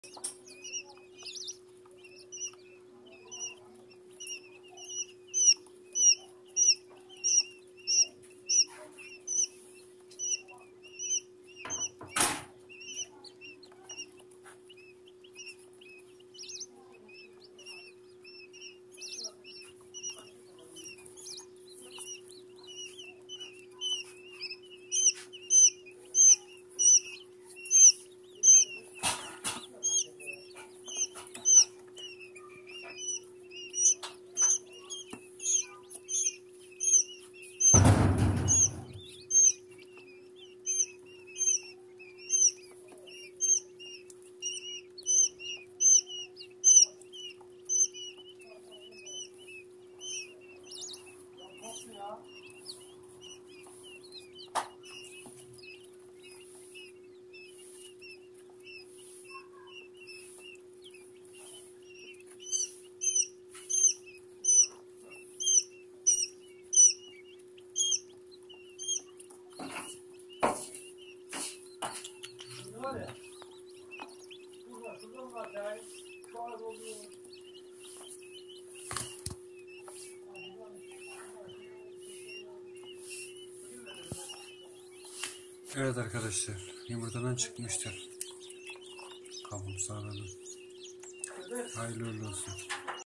BILDIRCIN YUMURTA'DAN ÇIKIM ANI🤗
Sevimli miniklerim çıktı çok şükür geri kalanıda yolda.